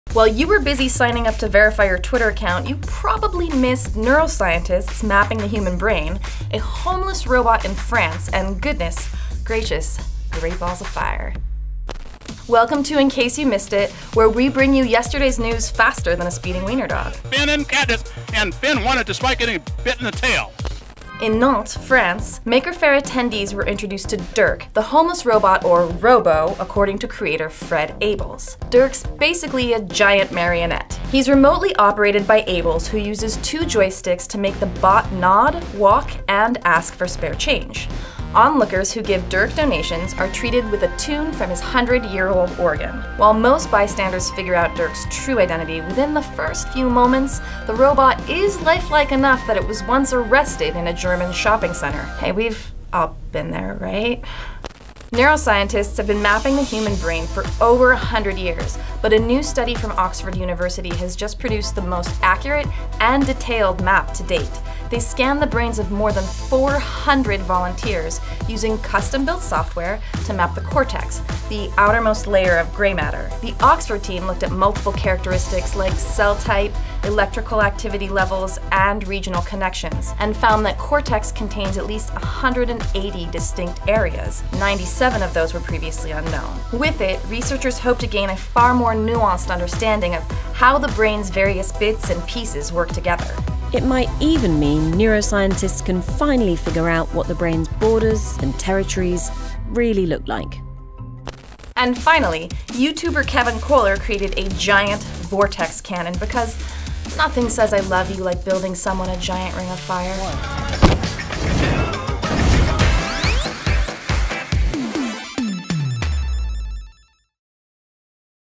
ICYMI: Homeless robots, modern brain maps and giant vortex cannons
The robot thing isn't as sad as it sounds.